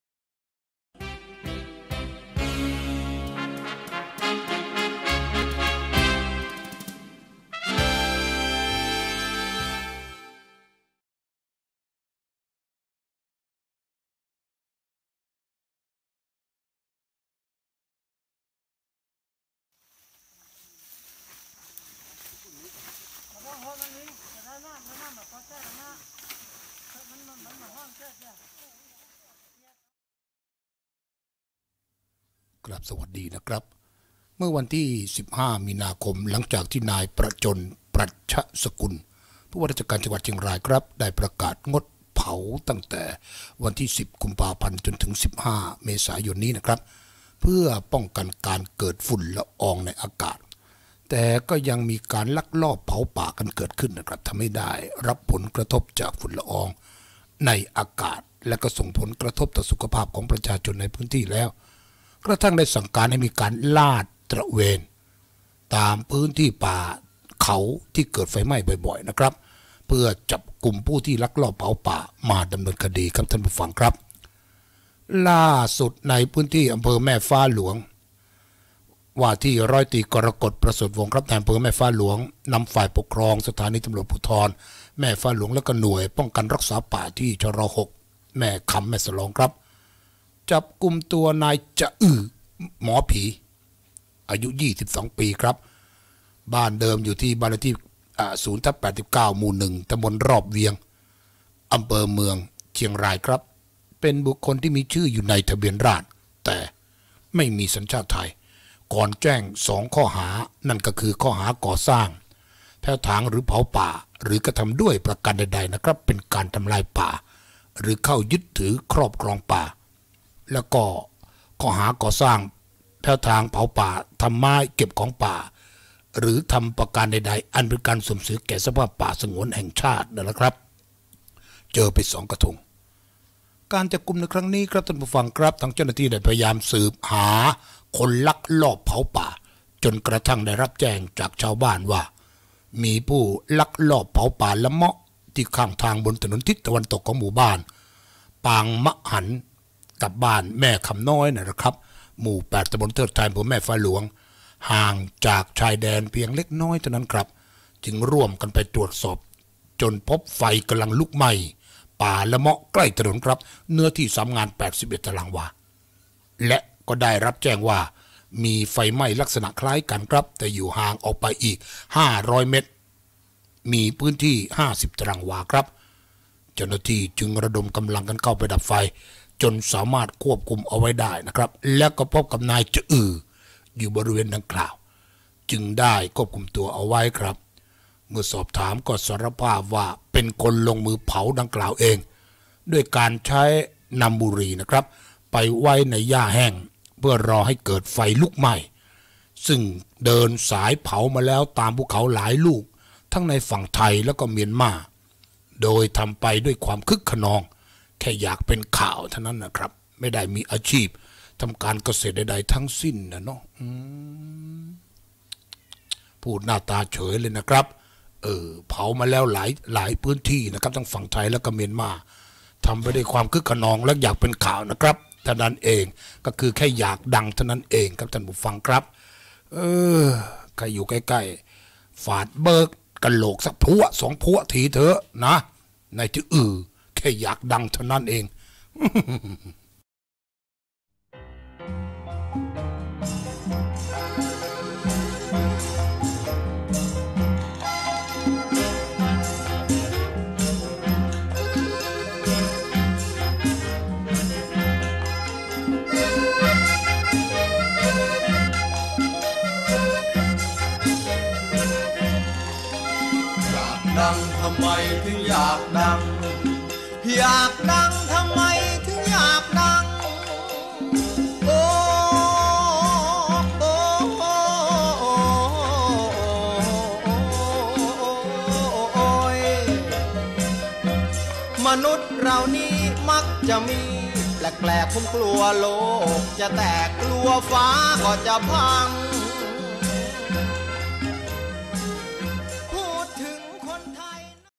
ฟังมันพูด!! เผาป่าเเค่อยากดังหวังเป็นข่าว!!...พลังภูผา
..ข่าวพลังภูผา..
ฟังมันพูด!! เผาป่าเเค่อยากดังหวังเป็นข่าว!!
เมื่อวันที่ 15 มี.ค. หลังจากที่ นายประจญ ปรัชสกุล ผวจ.เชียงราย ได้ประกาศงดเผาตั้แต่วันที่ 10 ก.พ.- 15 เม.ย. เพื่อป้องกันการเกิดฝุ่นละอองในอากาศ แต่ก็ยังมีการลักลอบเผาป่าเกิดขึ้น ทำให้ได้รับผลกระทบจากฝุ่นละอองในอากาศและส่งผลกระทบตอสุขภาพของประชาชนในพื้นที่แล้ว กระทั่งได้สั่งการให้มีการลาดตระเวณตามพื้นที่ป่าเขาที่เกิดไฟไหม้บ่อยครั้ง เพื่อจับกุมผู้ที่ลักลอบเผาป่ามาดำเนินคดี

ล่าสุด ในพื้นที่ อ.แม่ฟ้าหลวง ว่าที่ ร.ต.กรกฎ ประเสริฐวงษ์ นอภ.แม่ฟ้าหลวง นำฝ่ายปกครอง สภ.แม่ฟ้าหลวง และหน่วยป้องกันรักษาป่าที่ ชร.6 (แม่คำ-แม่สลอง) จับกุม นายจะอื่อ หมอผี อายุ 22 ปี บ้านเลขที่ 0/89 หมู่ 1 ต.รอบเวียง อ.เมืองเชียงราย เป็นบุคคลที่มีชื่ออยู่ในทะเบียนราษฏร์ แต่ไม่มีสัญชาติไทย ก่อนแจ้ง 2 ข้อหาคือข้อหาก่อสร้าง แผ้วถางหรือเผาป่า หรือกระทำด้วยประการใดๆ อันเป็นการทำลายป่าหรือเข้ายึดถือครอบครองป่า และข้อหาก่อสร้าง แผ้วถาง เผาป่า ทำไม้ เก็บของป่า หรือกระทำประการใดๆ อันเป็นการเสื่อมเสียแก่สภาพป่าสงวนแห่งชาติ

อการจับกุมครั้งนี้เจ้าหน้าที่ได้พยายามสืบหาคนลักลอบเผาป่า จนกระทั่งได้รับแจ้งจากชาวบ้านว่ามีผู้ลักลอบเผาป่าละเมาะที่ข้างทางบนถนนทิศตะวันตกของหมู่บ้านปางมะหัน-บ้านแม่คำน้อย หมู่ 8 ต.เทอดไทย อ.แม่ฟ้าหลวง ห่างจากชายแดนเพียงเล็กน้อย จึงร่วมกันไปตรวจสอบจนพบไฟลุกไหม้ป่าละเมาะใกล้ถนน เนื้อที่ 3 งาน 81 ตารางวา และก็ได้รับแจ้งว่ามีไฟไหม้ลักษณะคล้ายกัน แต่อยู่ห่างออกไปอีก 500 เมตร มีพื้นที่ 50 ตารางวา

เจ้าหน้าที่จึงระดมกำลังกันเข้าดับไฟจนควบคุมเอาไว้ได้และพบนายจะอื่ออยู่บริเวณดังกล่าว จึงได้ควบคุมตัวเอาไว้ เมื่อสอบถามก็สารภาพว่าเป็นคนลงมือเผาดังกล่าวเองด้วยการใช้นำบุหรี่ไปไว้ในหญ้าแห้งเพื่อรอให้เกิดไฟลุกไหม้ ซึ่งเดินสายเผามาแล้วตามภูเขาหลายลูกทั้งในฝั่งไทยและเมียนมา โดยทำไปด้วยความคึกคะนองอยากเป็นข่าว ไม่ได้มีอาชีพทำการเกษตรใดๆทั้งสิ้น... Credit